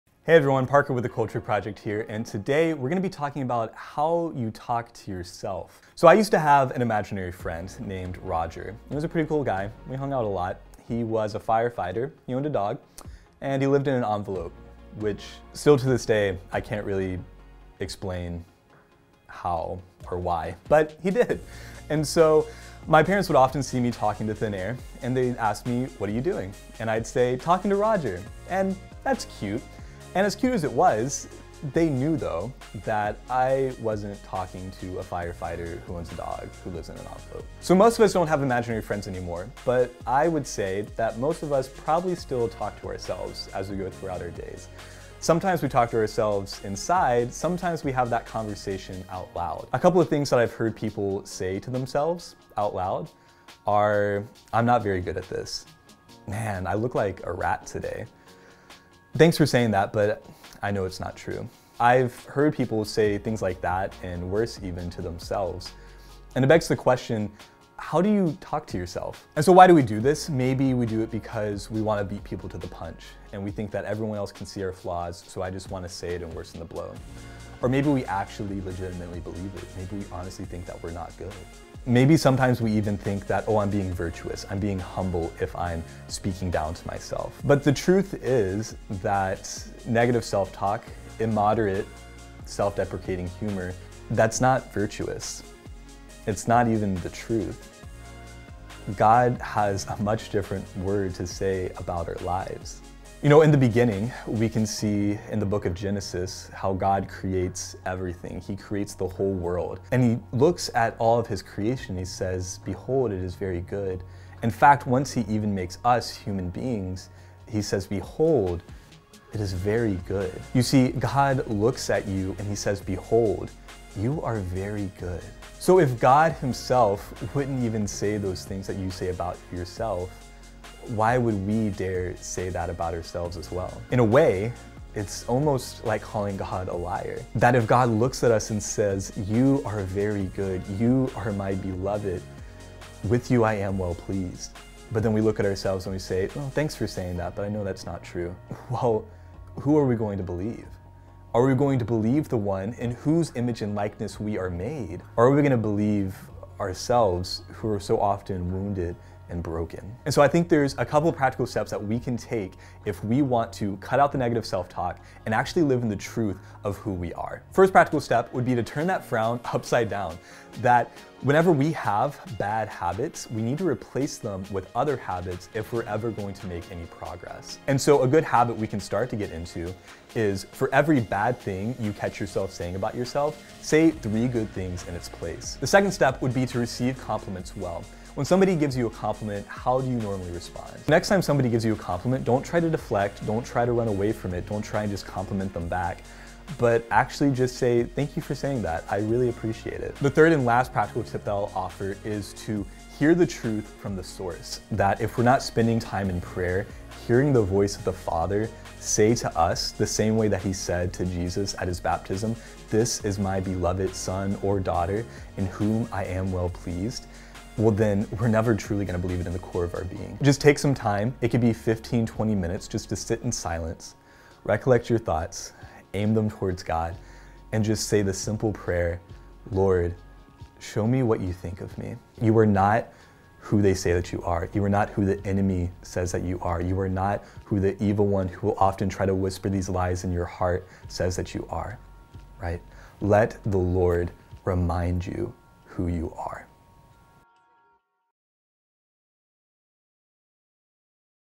How Negative Self Talk Calls God a Liar
In the beginning, God looked at all He created and said "It is very good". You are part of that "very good" creation.

When we talk bad about ourselves we may think we're being humble, but there is no humility in that. Oftentimes, we are speaking a different word than the one the Lord has spoken about ourselves.